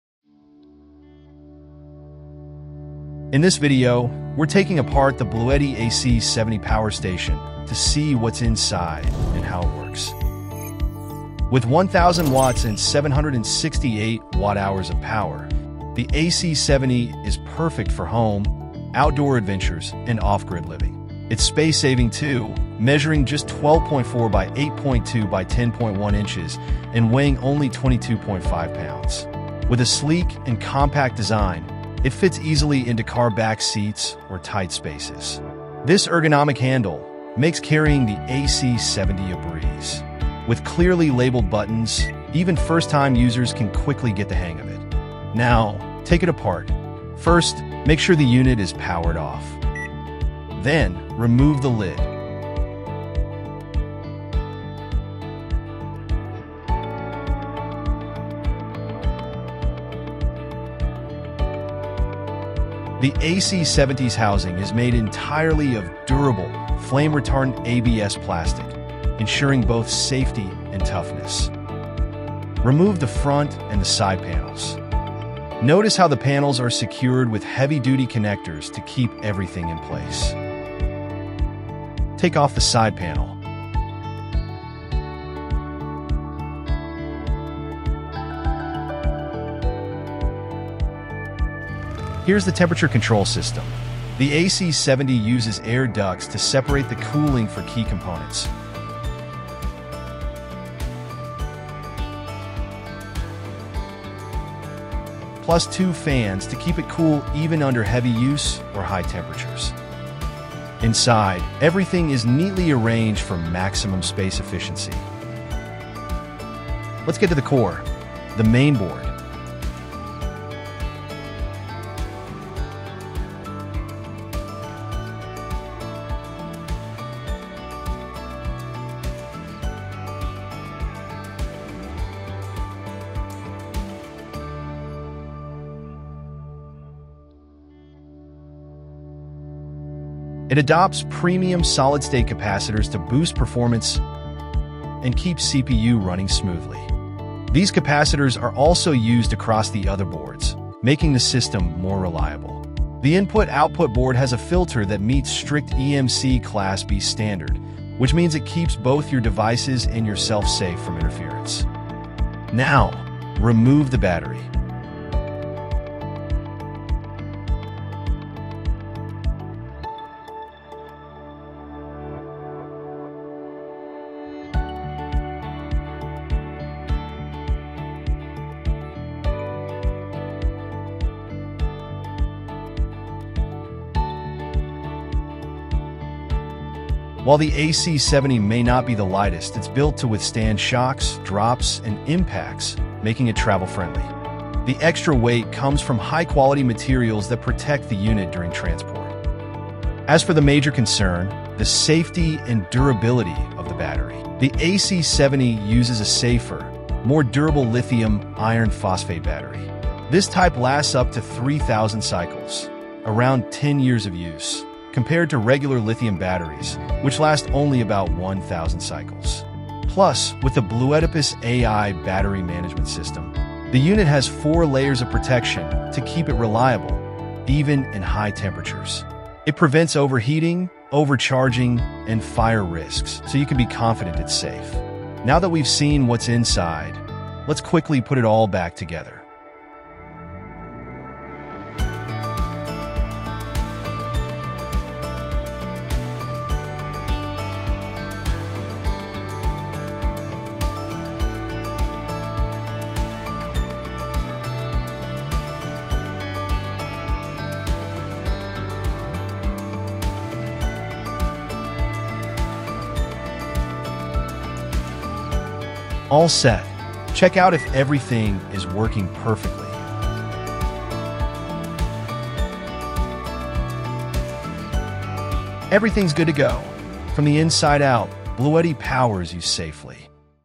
BLUETTI AC70 Teardown | Uncovering the Power Inside
In this video, we tear down the BLUETTI AC70 power station to take a closer look at its design and features. See how this compact and reliable energy solution is built to handle your power needs, whether at home or on the go. Watch the full teardown to get an inside view of what makes the AC70 a powerful choice for off-grid living and more!

0:00 - Introduction
0:51 - Remove the Lid
1:12 - Remove the Front and the Side Panels
1:33 - Remove the Air Duct Paper and Fans
1:57 - Remove the Motherboard
2:43 - Remove the Battery
3:56 - Reassembly & Testing

* Do not disassemble at home. Risk of laser exposure, electric shock, or other injury. Opening the product voids the warranty.
（Creator Pro License Number - Xe2FuH）